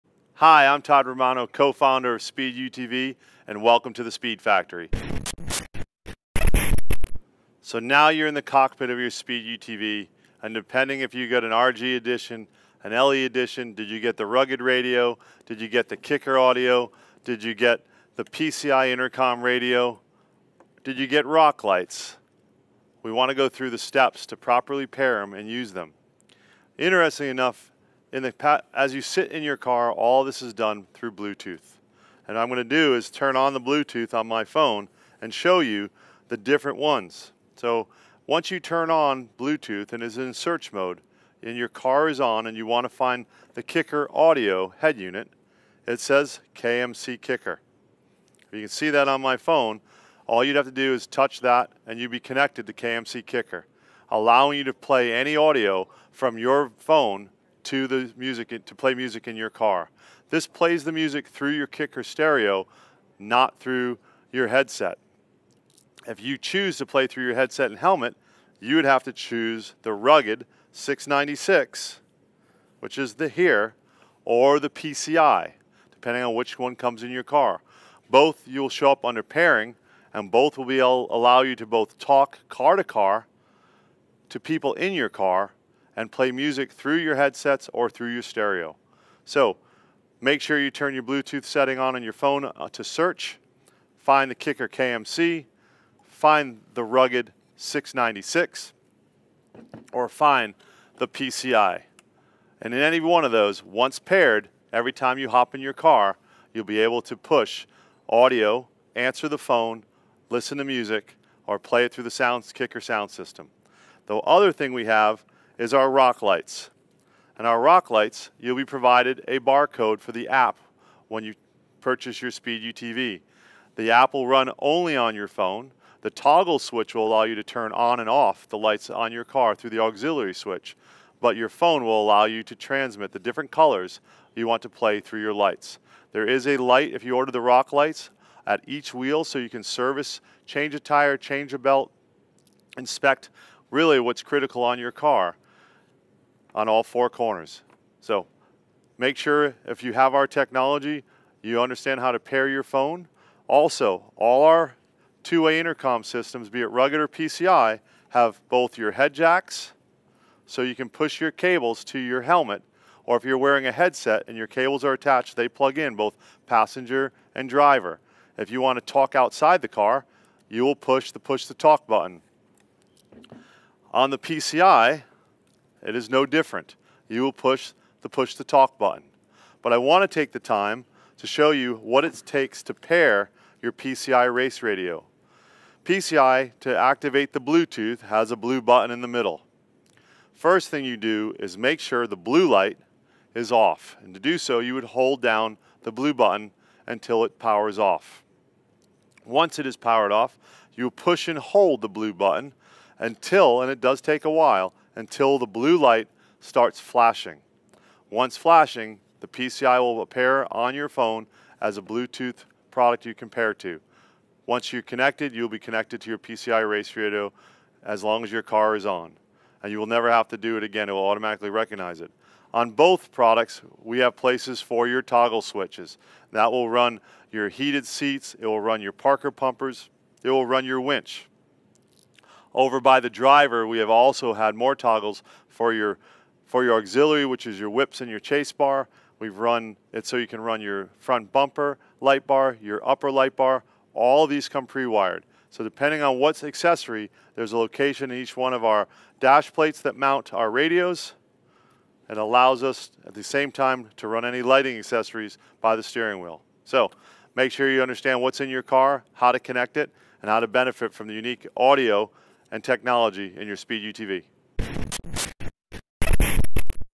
Speed UTV Radio/Intercom
Speed UTV Radio and intercom kit.  Learn about the unique design, functionalitiy and ease of instal of the Speed UTV Radio/Intercom kit.  Learn about your two different options your have with both Rugged Radio's and PCI.